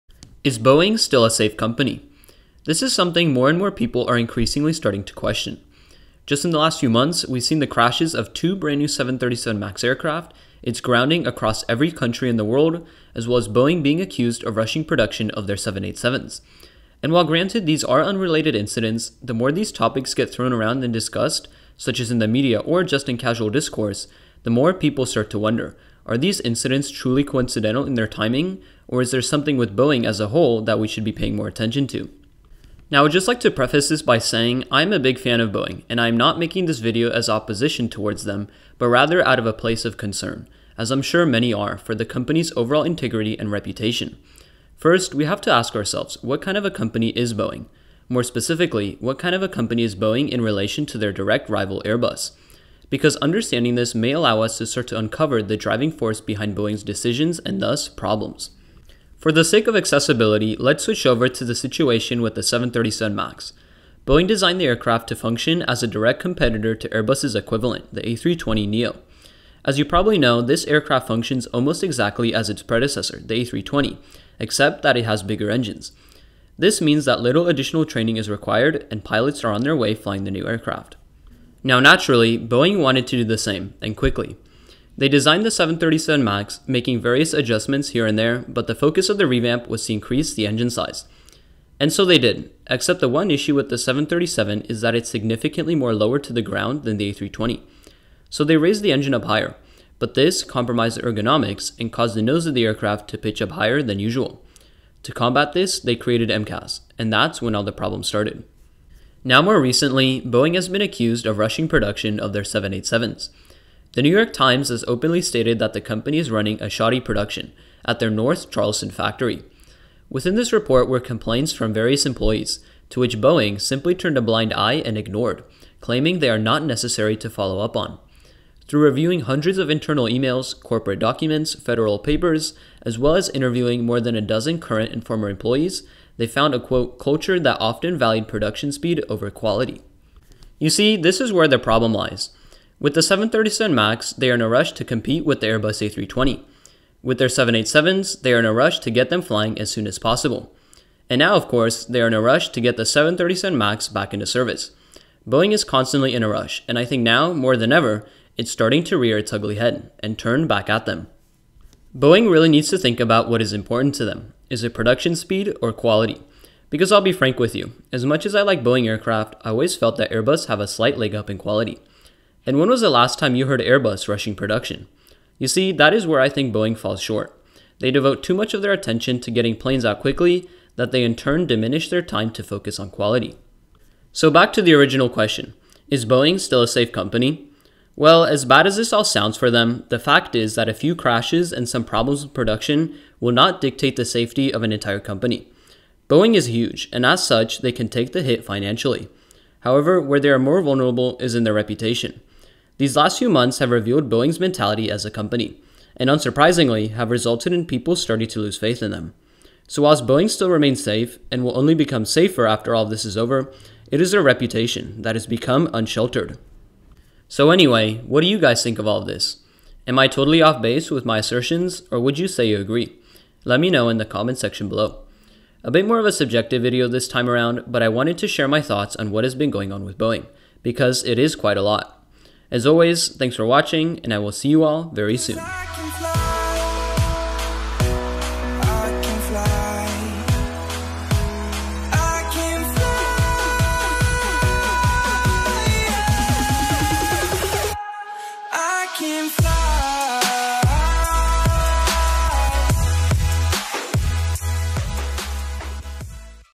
Is Boeing Still a Safe Company?
• AviationToday Series

In this episode, we discuss Boeing's integrity as a company, their recent issues with the 737 MAX and 787 production, their reputation, as well as my personal thoughts on the matter. Enjoy the video, and have a great day!

Airbus
Boeing

(I will try my best to respond to you as soon as possible.)

• Created using iMovie
• Pure Aviation 2019